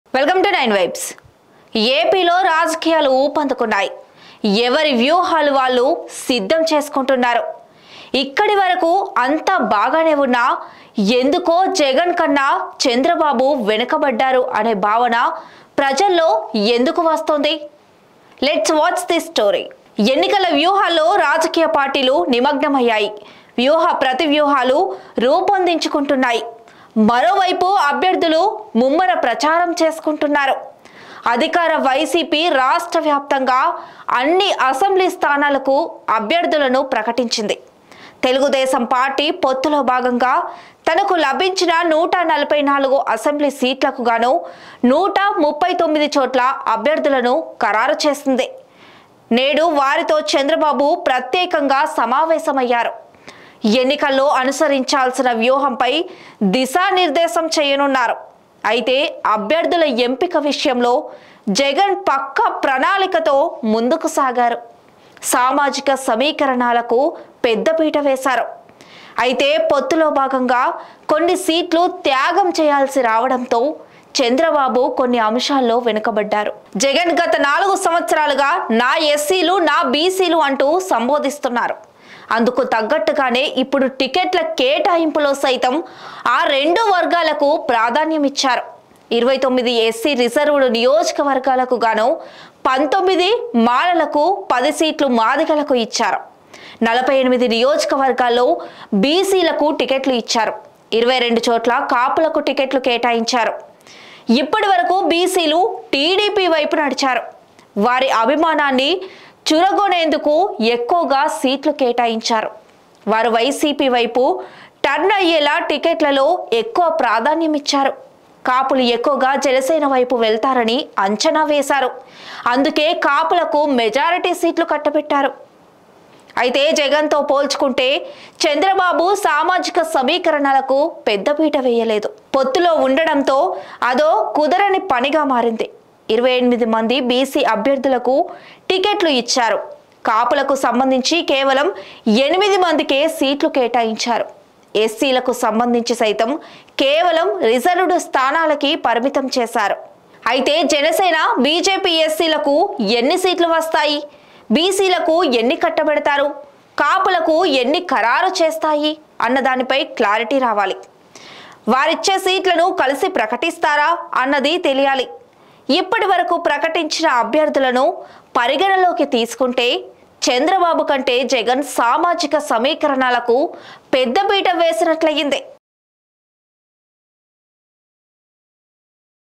చంద్రబాబు ఎందుకో వెనకపడ్డారు..! || YCP vs TDP campaign tactics || Chandrababu vs Jagan
As politics gains momentum in Andhra Pradesh, attention turns to the strategies being formulated by political parties. While the ruling YCP and TDP finalize their candidates and campaign tactics, questions arise about Chandrababu's perceived lag behind Jagan. Despite alliances, Jagan's meticulous planning and focus on social mobilization seem to outshine Chandrababu's approach. Explore the dynamics shaping AP's political landscape in this intriguing story.